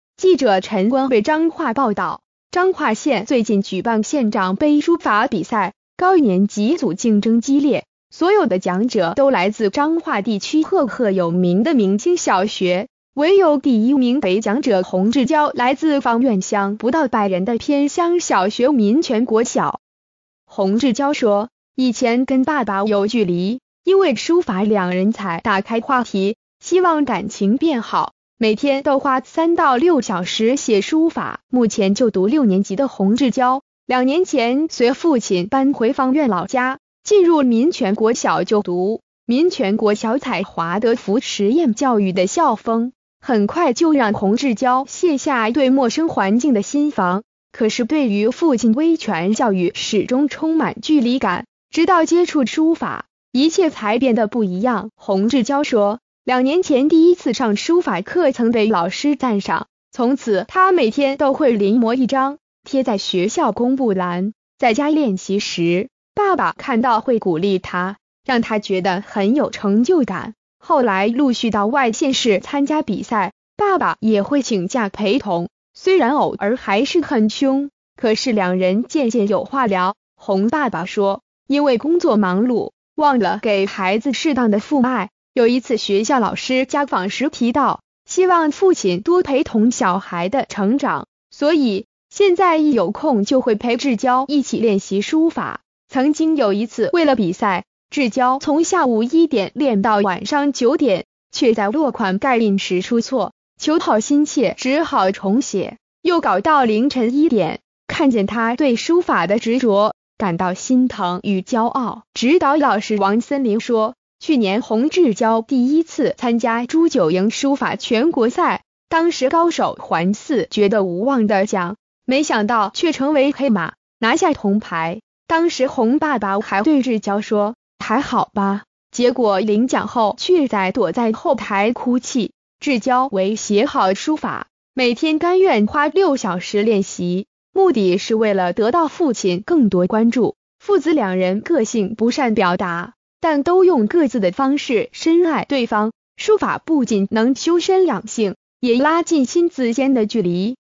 「想跟爸爸感情變好」 書法小將日練6小時奪冠
〔記者陳冠備／彰化報導〕彰化縣最近舉辦縣長盃書法比賽，高年級組競爭激烈，所有得獎者都來自彰化地區赫赫有名的明星小學，唯有第一名得獎者洪稚蛟來自芳苑鄉不到百人的偏鄉小學民權國小。洪稚蛟說，以前跟爸爸有距離，因為書法兩人才打開話題，希望感情變好，每天都花3到6小時寫書法！

發表評論評論如下。